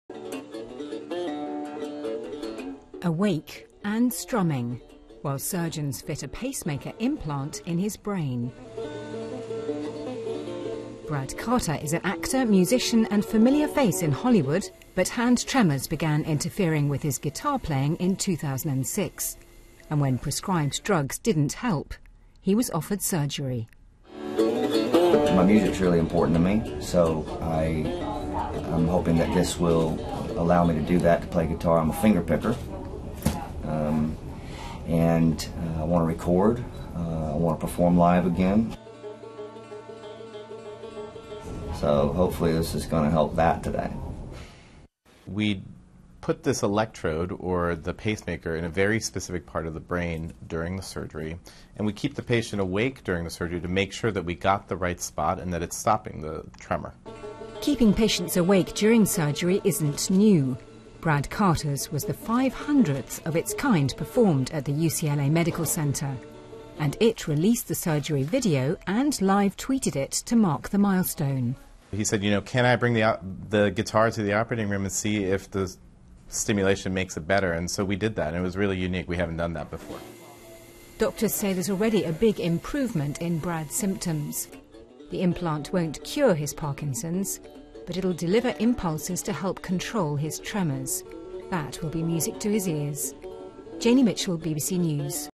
Man plays guitar during brain surgery to treat Parkinson's
In a medical first, a patient has played the guitar while undergoing brain surgery in the US.

The aim was to help surgeons pinpoint exactly where to place electrodes to treat the effects of his Parkinson's disease, as Janey Mitchell explains.